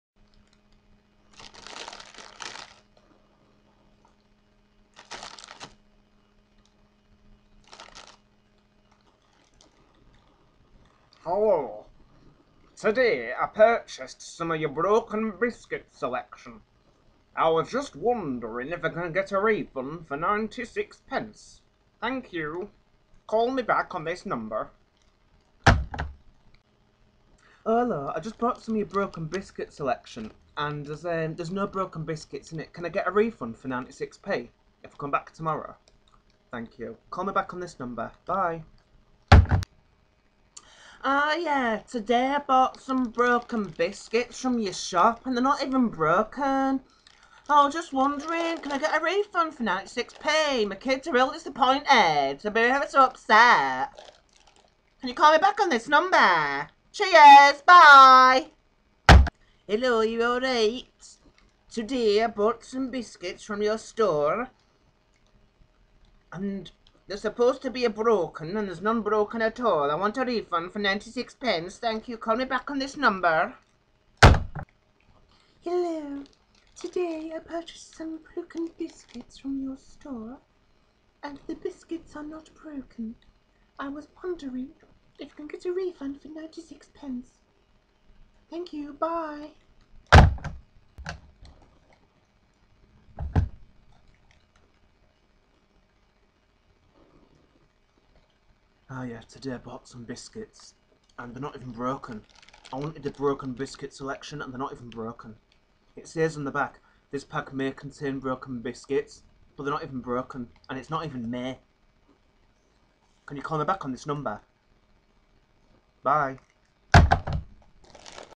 I do voices 1 (the non broken biscuits)
The first six voices. These were supposed to be broken biscuits!